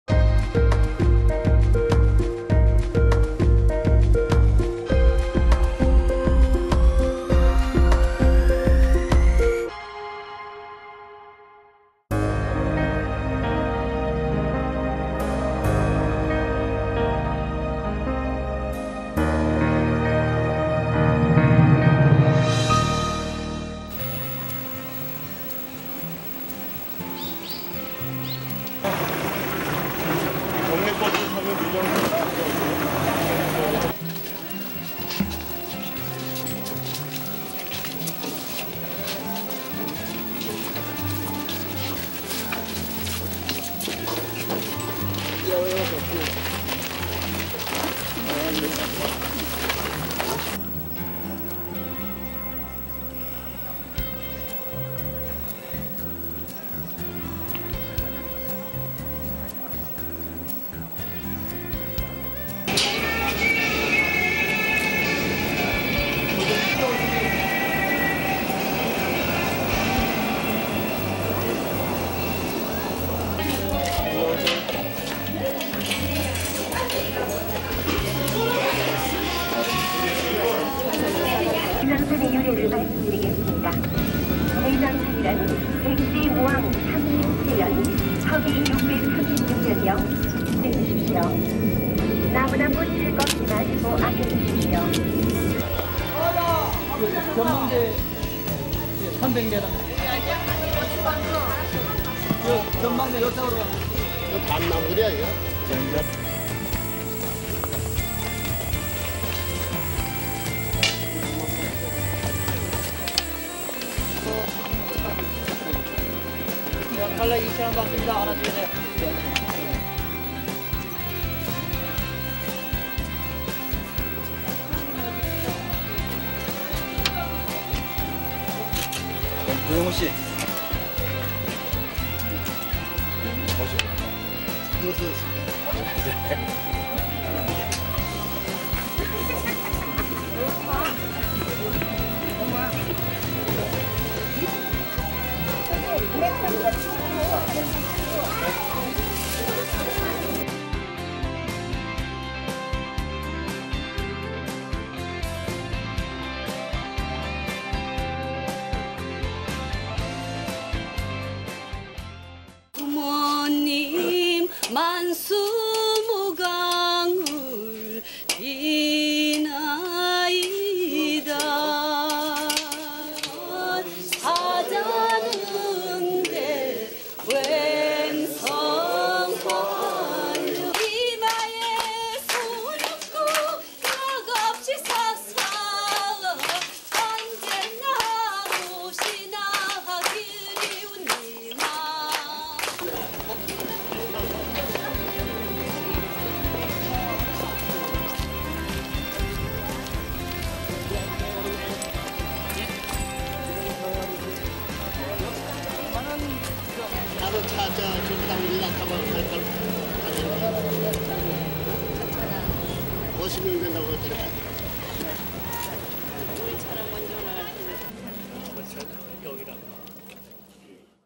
[섭리의 보물창고 디지털 아카이브] 1987년 10월 26일 참부모님 내장산 나들이
1987년 10월 26일 참부모님 내장산 나들이 (전라북도 정읍)

더 많은 영상을 만나고 싶다면 아래 홈페이지 Link를 클릭해주세요.